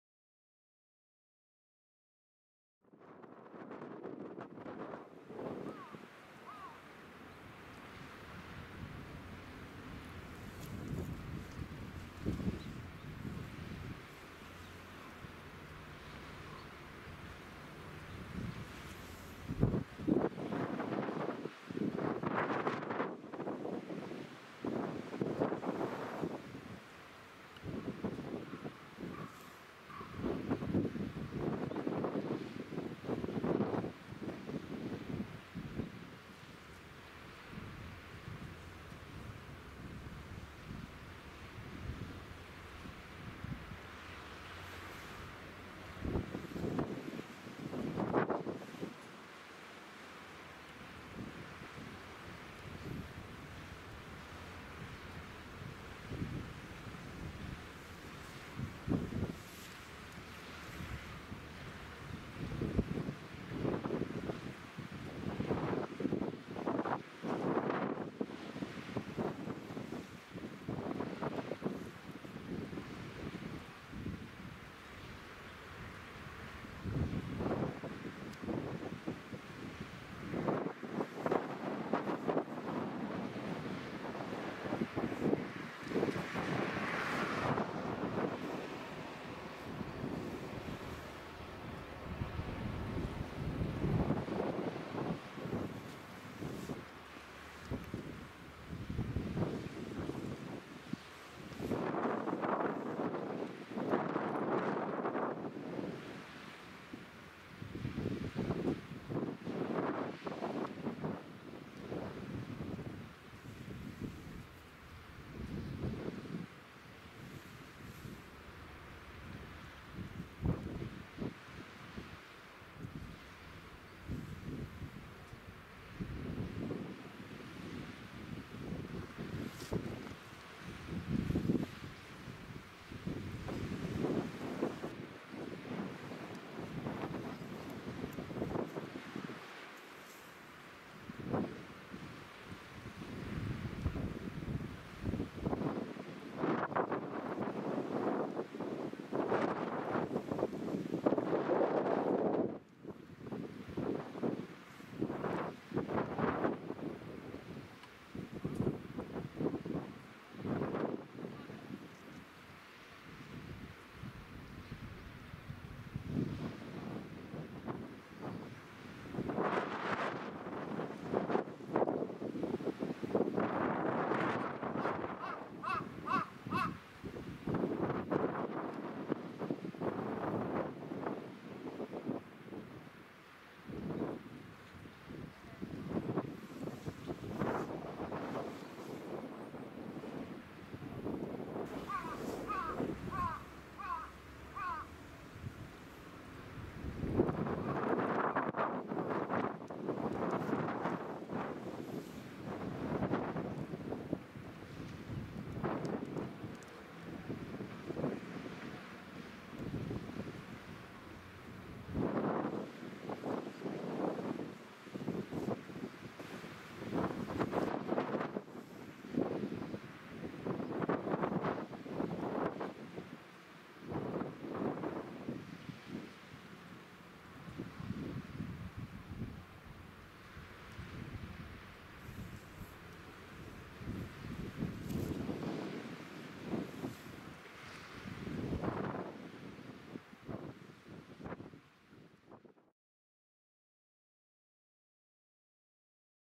遠ざかる巡視船「きたかみ」 令和4年1月24日
巡視船「きたかみ」が、向島の造船所での整備点検を終えて釜石へ帰って行きました。